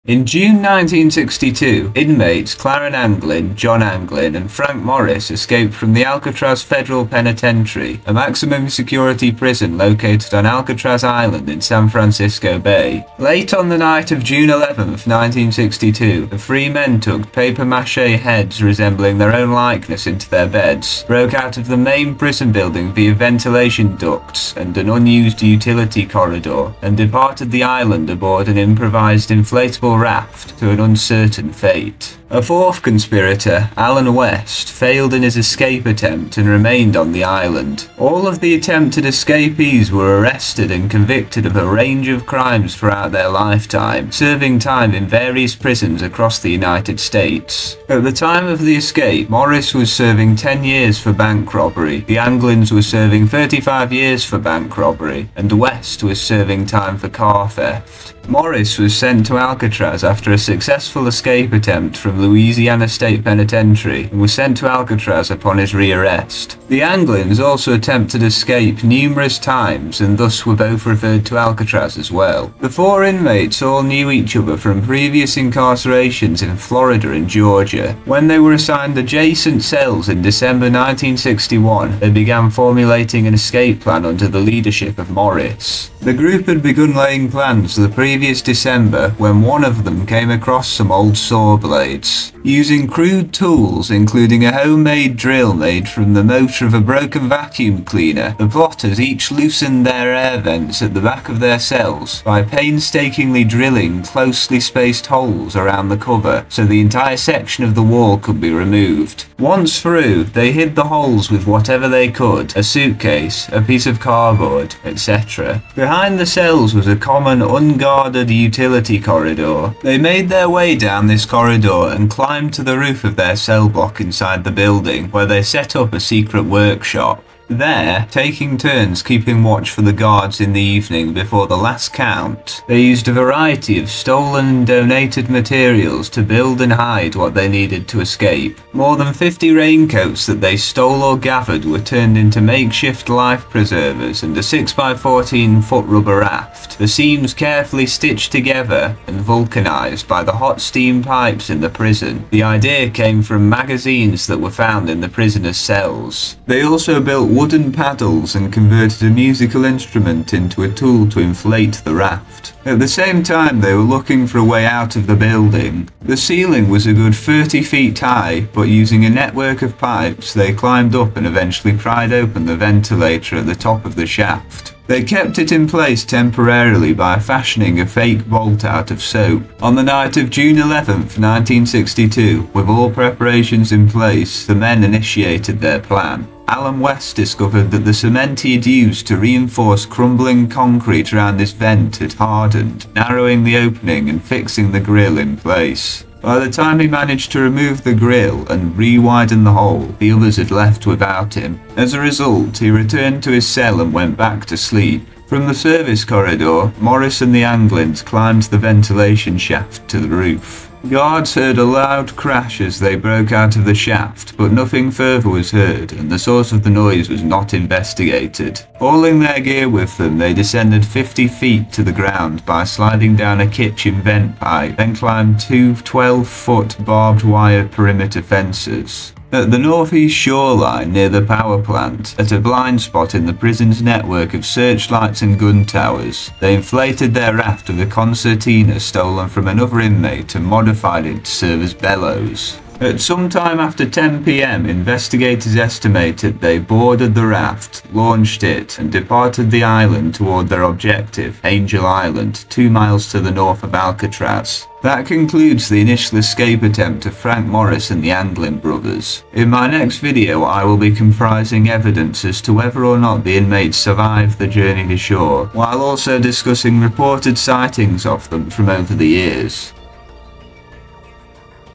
ESCAPE FROM ALCATRAZ PRISON EXPLAINED - How 3 men escaped the "unescapable" prison
The Alcatraz Prison Escape of June 1962 was an escape attempt by three inmates of the Alcatraz Federal Penitentiary in San Francisco, California. The inmates, Frank Morris, John Anglin, and Clarence Anglin, used improvised tools to carve their way out of their cells and used a makeshift raft to traverse the San Francisco Bay in an attempt to reach the mainland.

Though they were never seen again, it is presumed that the trio perished in the attempt. The incident has become the subject of much speculation, with some believing that the men were successful in their escape. In this video I discuss how the men initiated the plan for and carried out the escape.